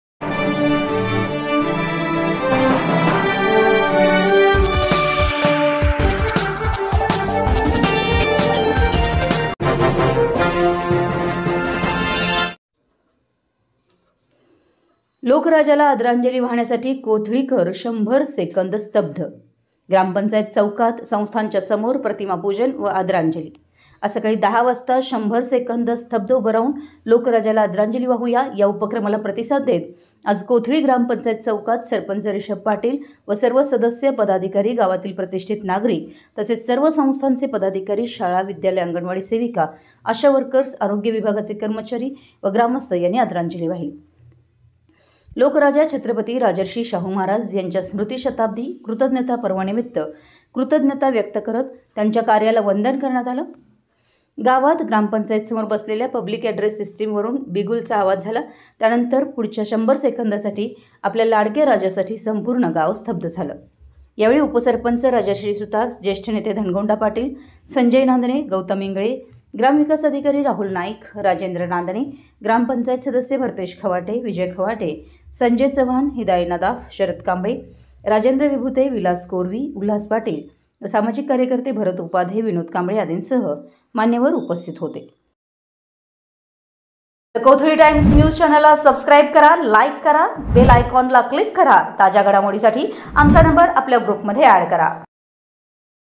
लोकराजाला आदरांजली वाहण्यासाठी कोथळीकर 100 सेकंद स्तब्ध
ग्रामपंचायत चौकात,   संस्थांच्या समोर प्रतिमा पूजन व आदरांजली